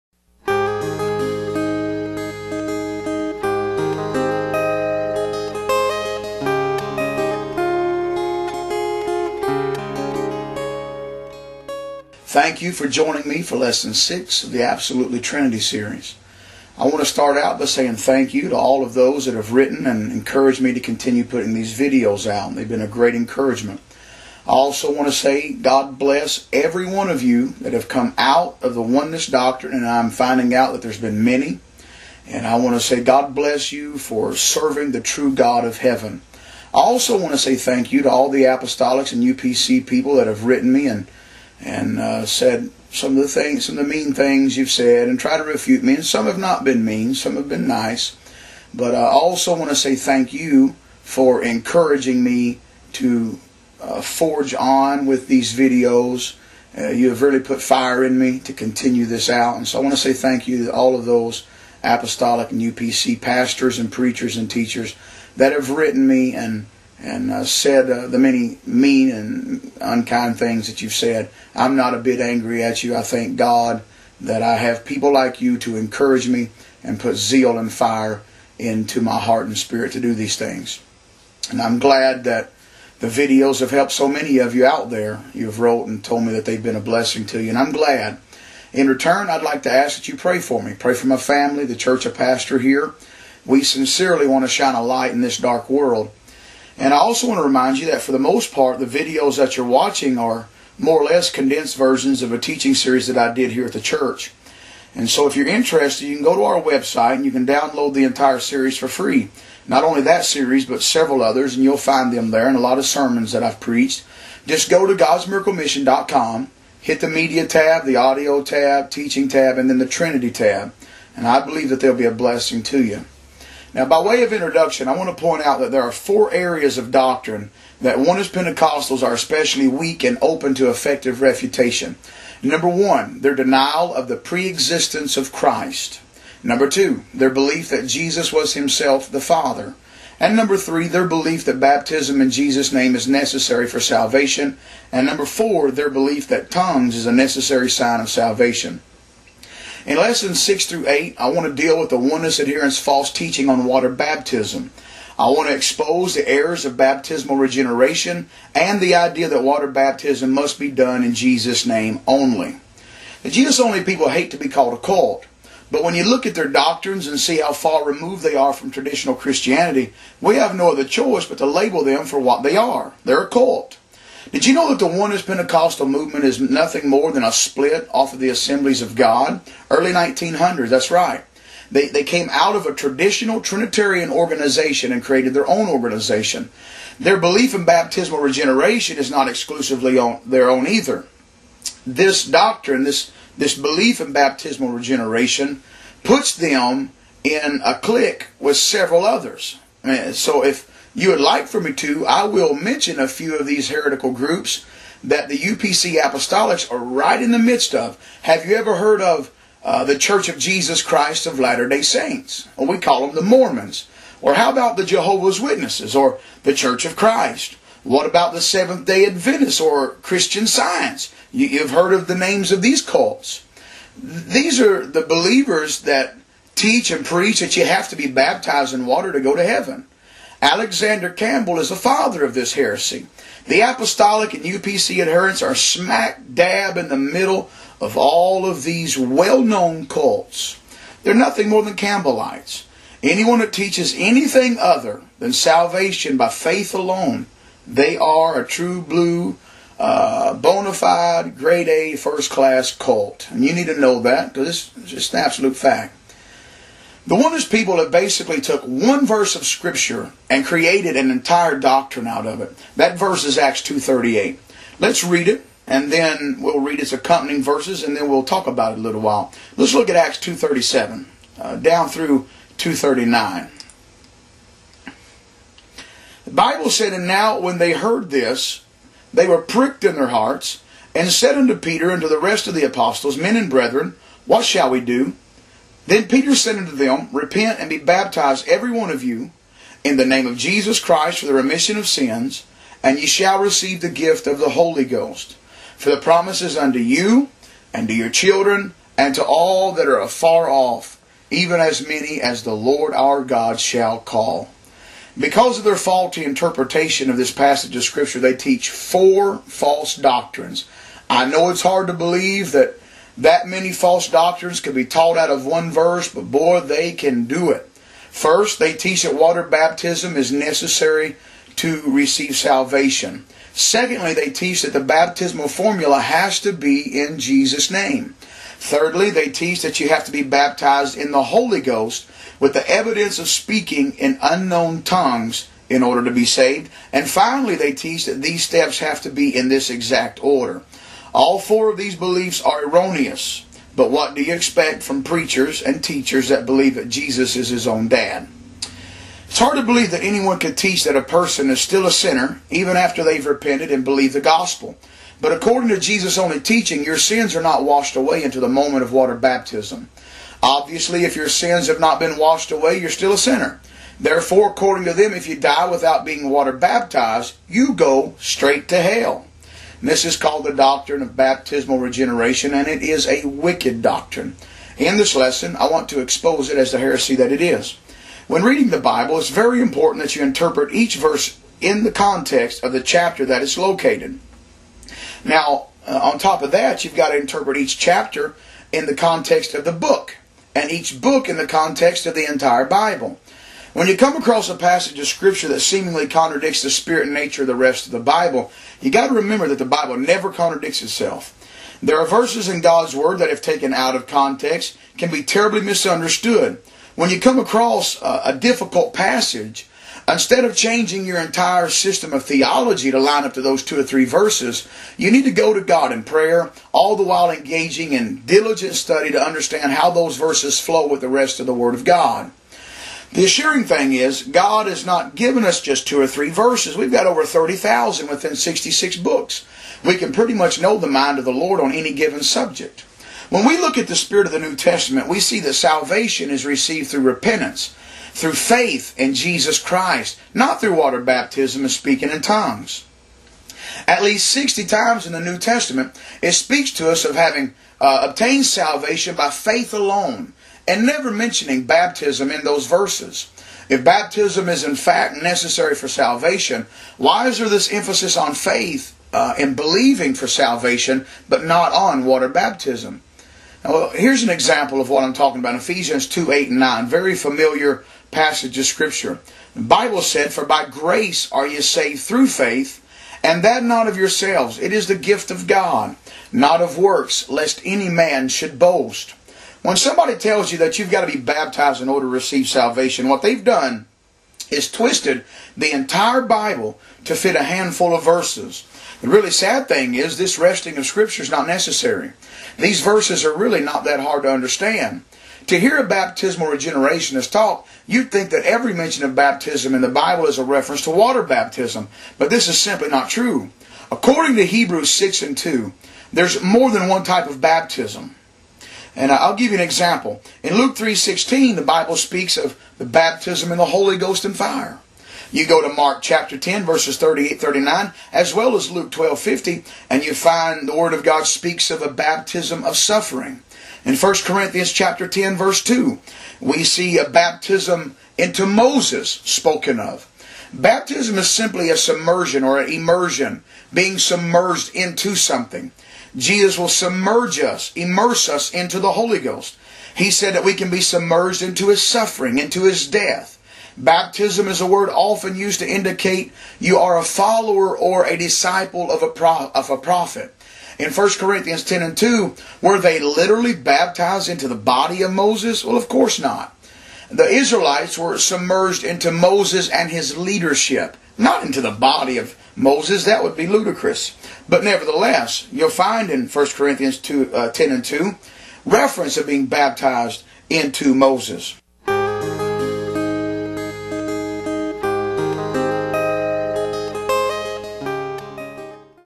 Trinity! Lesson 6 (Part 1) Errors of Baptismal Regeneration
This is Lesson 6 (Part 1) of Pastor D. L. Lamb's Absolutely Trinity! series. This lesson deals with the Errors of Baptismal Regeneration (the belief that a person must be baptized in water to be saved). This lesson also exposes the Oneness interpretation of Acts 2:38. A refutation of the Apostolic UPC Oneness Jesus Only Heresy Cult. David Lamb